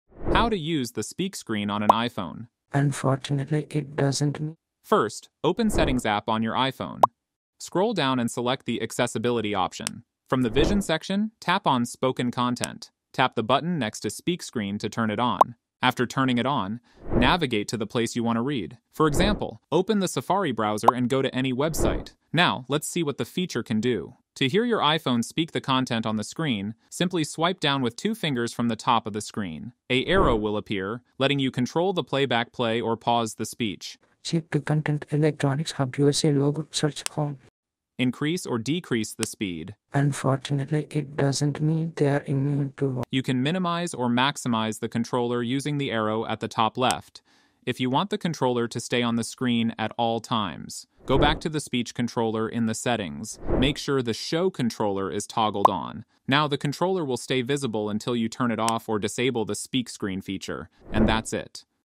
How to Use Speak Screen on iPhone?
In this comprehensive step-by-step guide, learn how to use the Speak Screen feature on your iPhone, a powerful accessibility tool designed to read aloud the text displayed on your screen. Whether you're looking to enhance your reading experience, assist those with visual impairments, or simply prefer auditory learning, this video will show you how to enable and customize Speak Screen settings effortlessly.

We'll cover:
- How to activate Speak Screen on your iPhone
- Adjusting speaking rate and voice options
- Tips for using Speak Screen effectively in different apps

By the end of this tutorial, you'll have all the knowledge you need to make the most out of this handy feature and enjoy a new way to consume content on your iPhone.